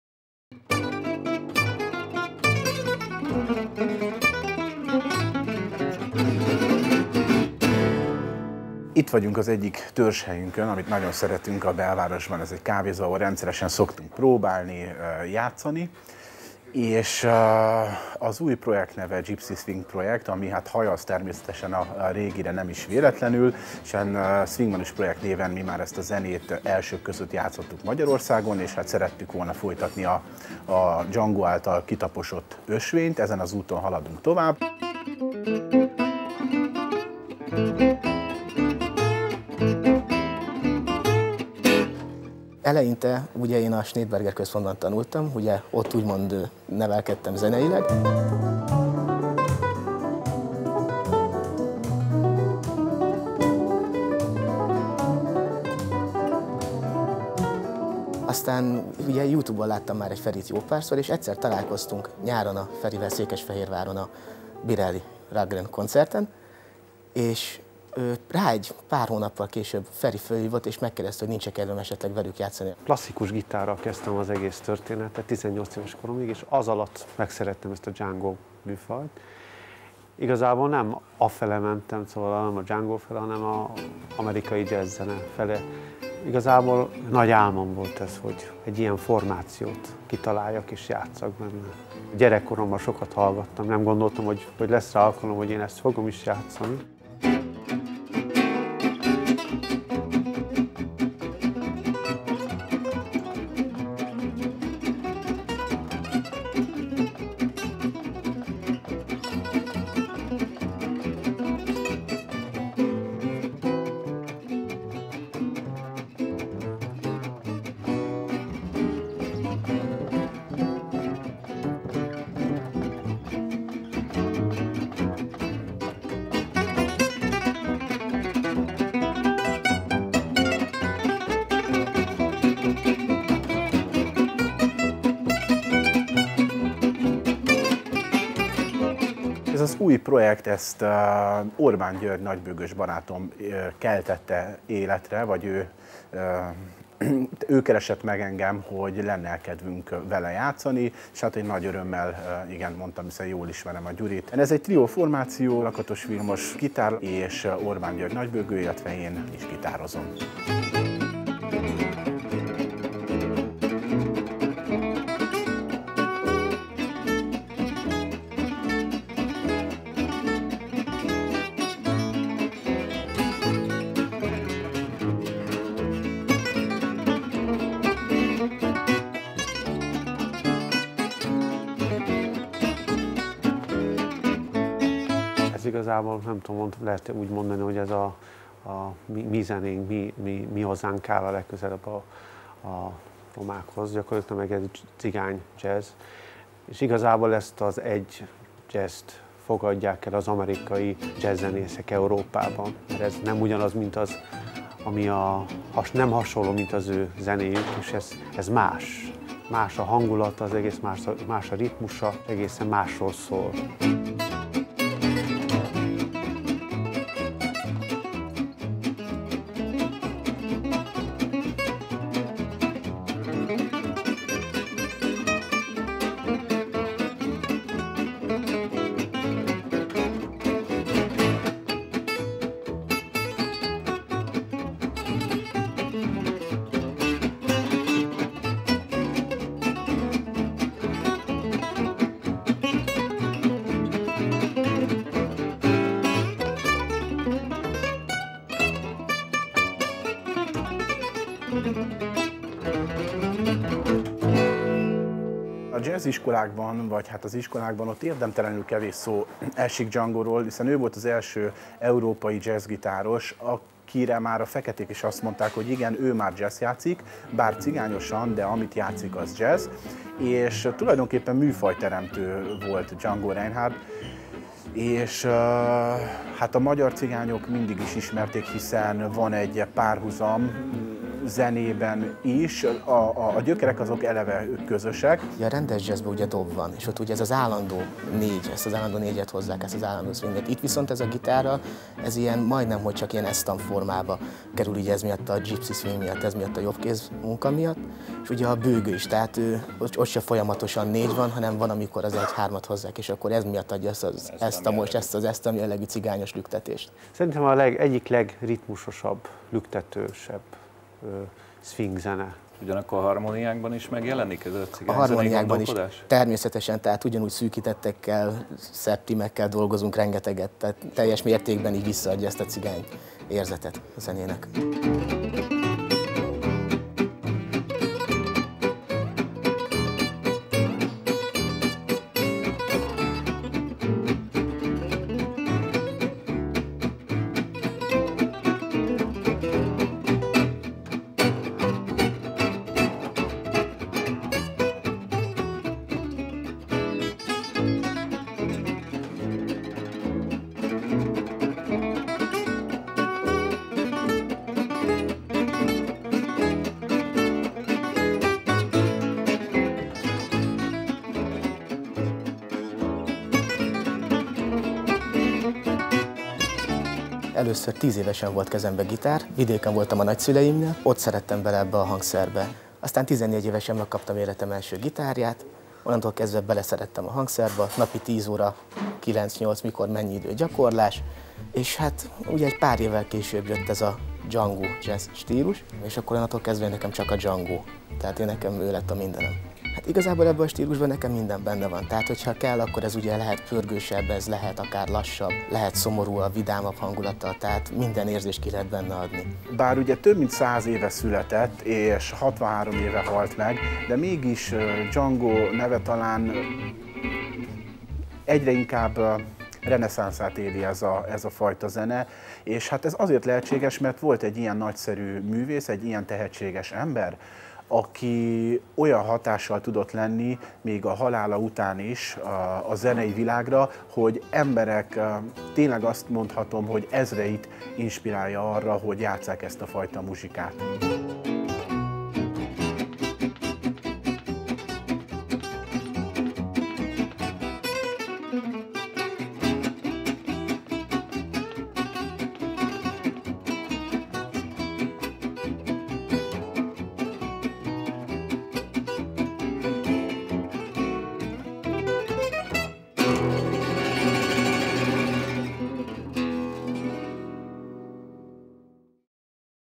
Hungarian gypsy jazz documentary with ENGLISH subtitle - Gipsy Swing Project (Budapest) -
Gypsy swing. This genre was created in the 1930’s  by a sinti gypsy called Django Reinhardt. Thanks to his genius, at the collision point of traditional gipsy music and American jazz an immortal music genre came to life. Even today, 63 years after Django’s death, his music is thriving all around the globe. One of the representatives of gipsy jazz in Hungary is the recently formed Gipsy Swing Project.
Ferenc Illes - guitar
Vilmos Lakatos - guitar
Gyorgy Orban - double bass